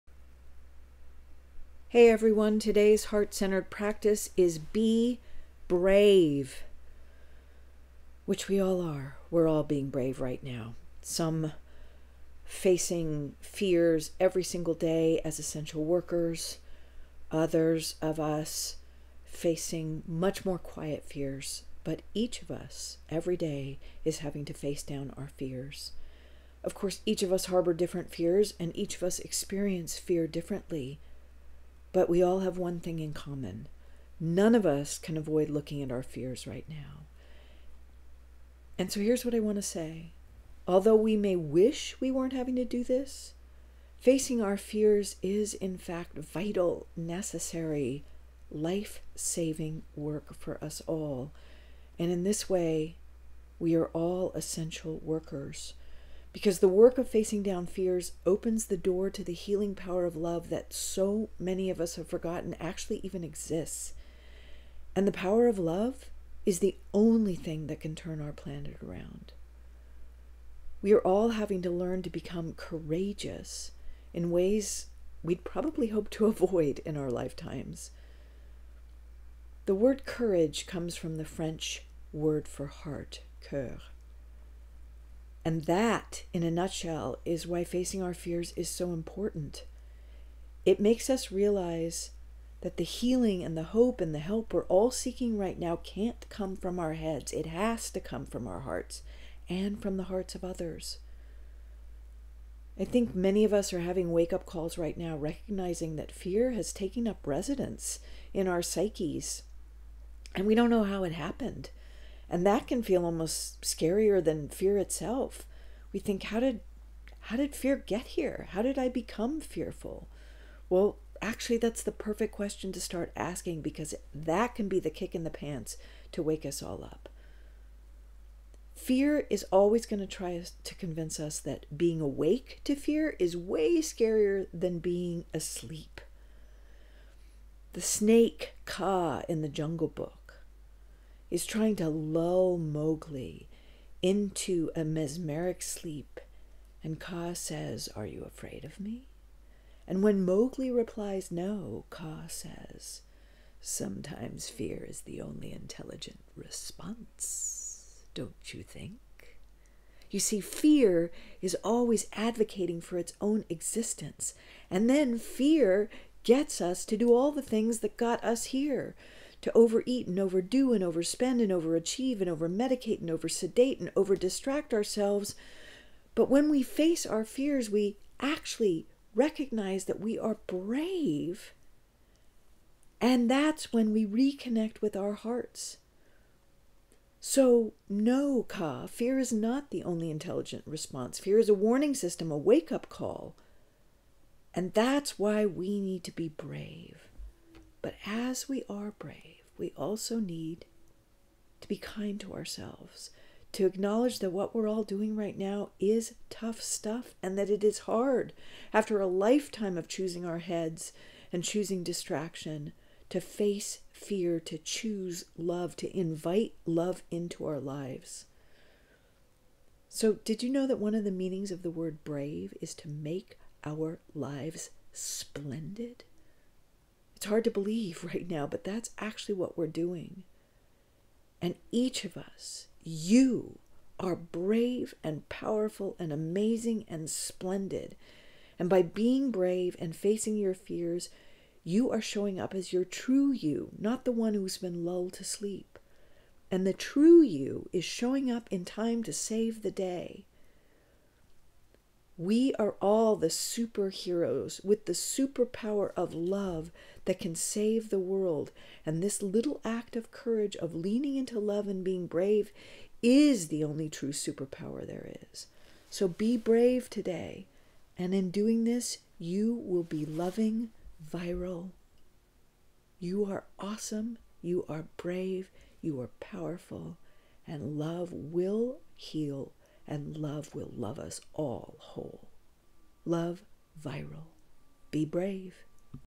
BE: Brave
We are all being called upon to be brave right now. But as you bravely face your fears, you need to be kind to yourself. Acknowledge to yourself that what you’re doing here is tough stuff. What you’re doing is very brave. Choosing your heart instead of your head. Choosing Love instead of fear. Facing your fears and inviting Love to heal them.
Did you know that one of the meanings of the word brave is “to make our lives splendid”?
Whenever we move out of our heads and into our hearts, whenever we begin to choose Love over fear, we begin to make our lives splendid. This is what you’re doing.
You are brave and powerful and amazing!